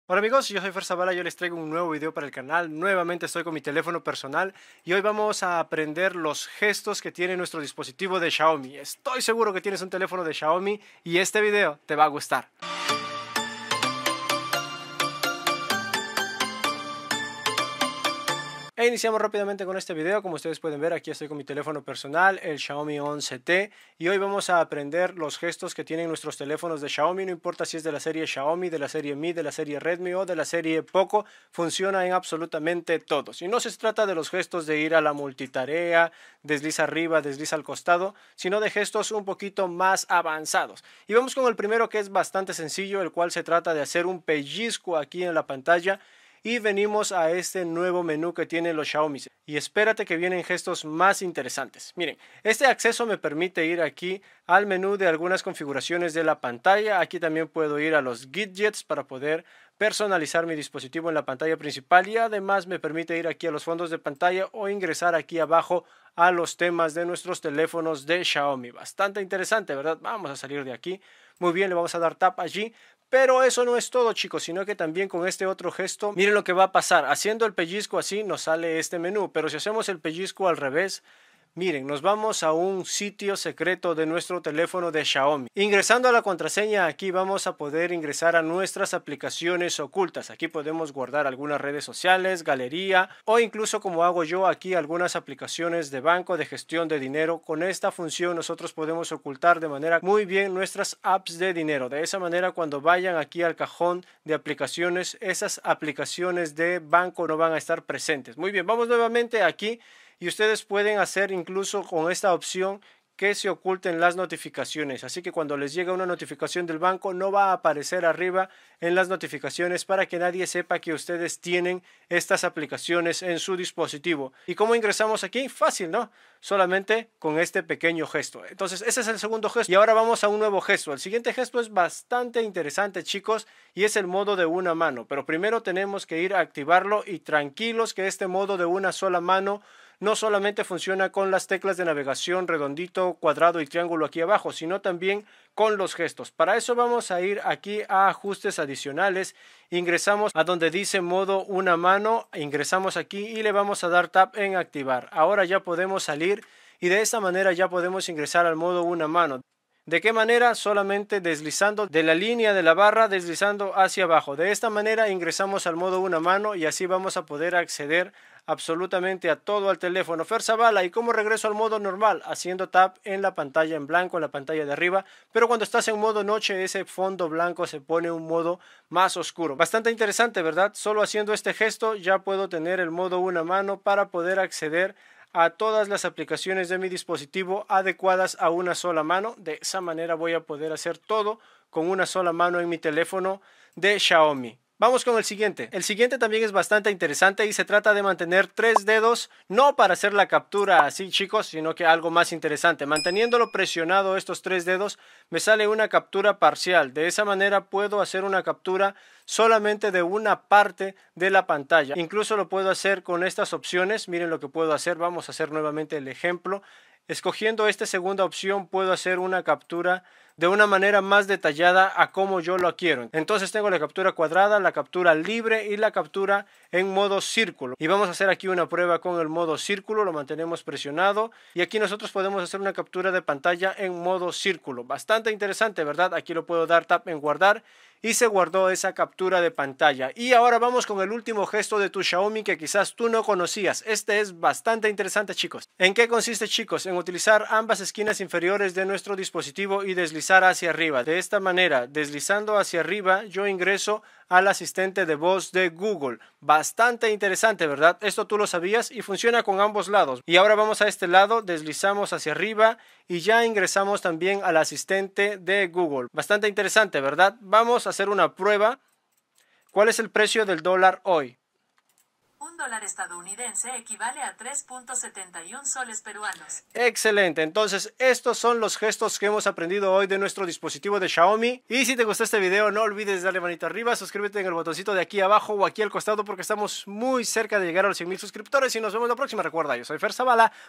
Top GESTOS que No sabías para tu XIAOMI
Hoy les traigo un top de los gesto para tu teléfono de Xiaomi que estoy seguro que te van a gustar!